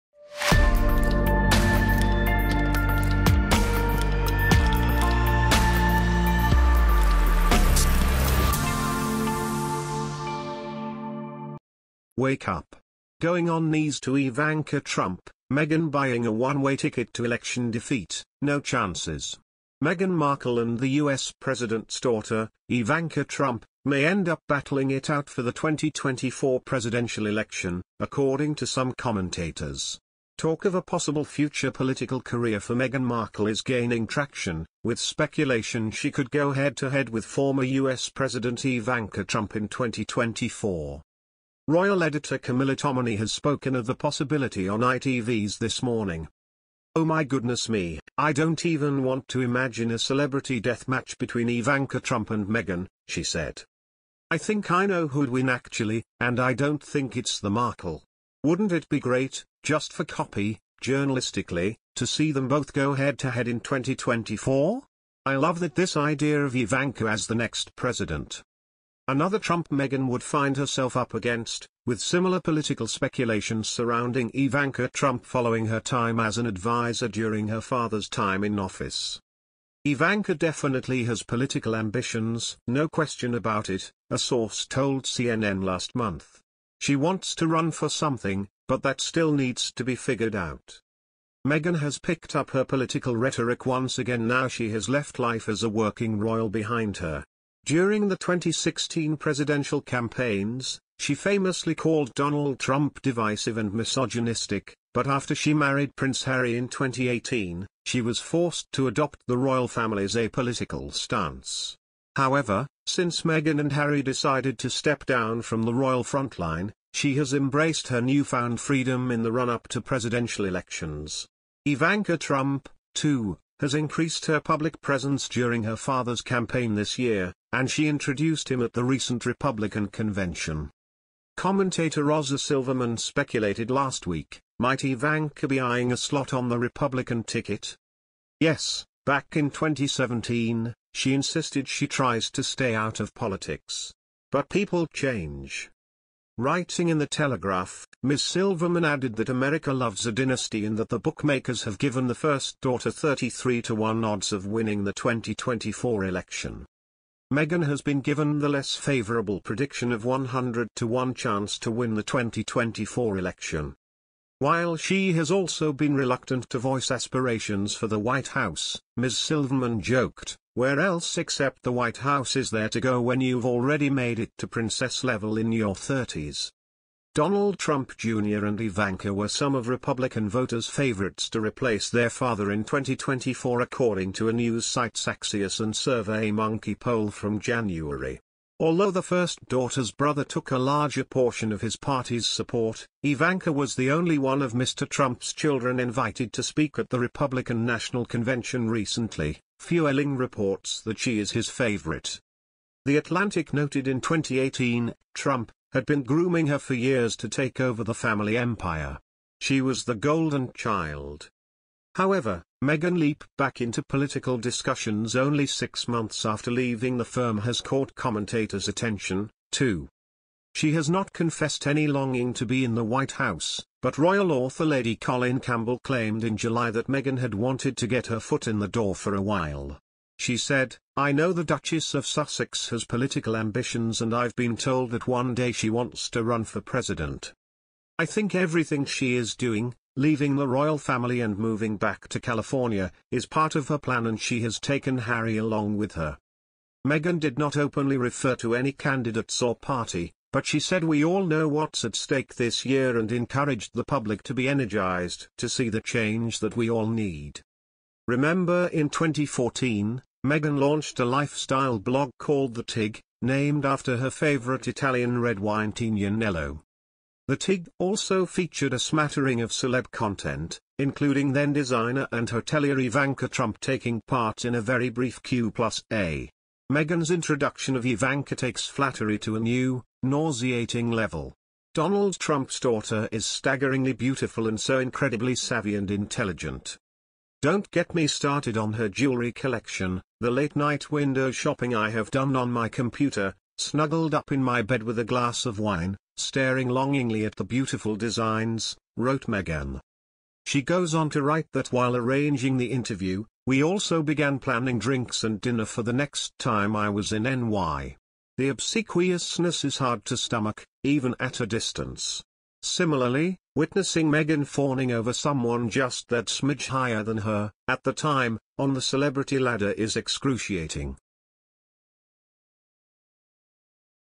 WAKE UP! Going ON KNEES To Ivanka Trump, Meg Buying A ONE-WAY TICKET To Election DEFEAT: NO CHANCES.
MEGHAN MARKLE and the US President's daughter, Ivanka Trump, may end up battling it out for the 2024 presidential election, according to some commentators.
Talk of a possible future political career for Meghan Markle is gaining traction, with speculation she could go head-to-head with former US President Ivanka  Trump in 2024.
Royal Editor Camilla Tominey has spoken of the possibility on ITV's This Morning.
"Oh my goodness me, I don't even want to imagine a celebrity deathmatch between Ivanka  Trump and Meghan," she said.
"I think I know who'd win actually, and I don't think it's the Markle.
"Wouldn't it be great, just for copy, journalistically, to see them both go head-to-head in 2024? I love that this idea of Ivanka  as the next President.
 another Trump Meghan would find herself up against, with similar political speculation surrounding Ivanka Trump following her time as an advisor during her father's time in office.
"Ivanka definitely has political ambitions, no question about it," a source told CNN last month. "She wants to run for something, but that still needs to be figured out."
Meghan has picked up her political rhetoric once again now she has left life as a working royal behind her. During the 2016 presidential campaigns, she famously called Donald Trump “divisive” and “misogynistic” — but after she married Prince Harry in 2018, she was forced to adopt the Royal Family’s apolitical stance. However, since Meghan and Harry decided to step down from the royal frontline, she has embraced her newfound freedom in the run-up to  presidential elections.